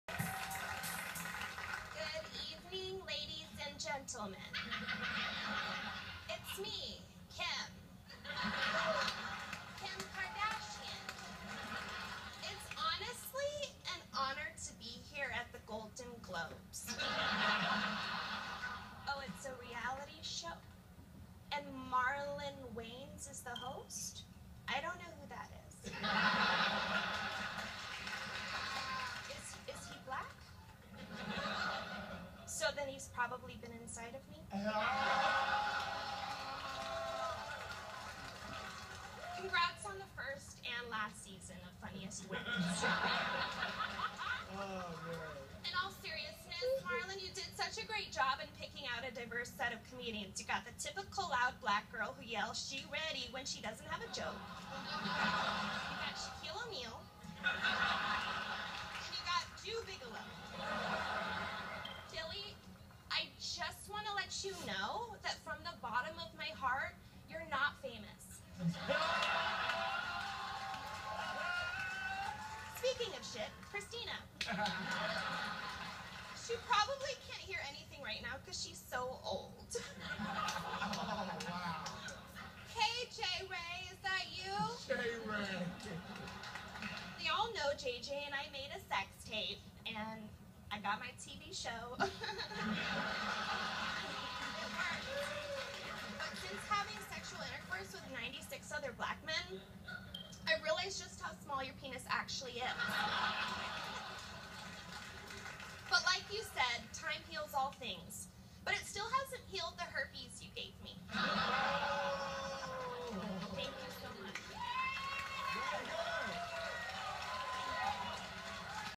Funniest Wins: Roast for Ray Jay. Manon Mathews
She won. SHE WAS THE FUNNIEST ONE OMG she played as Kim Kardashian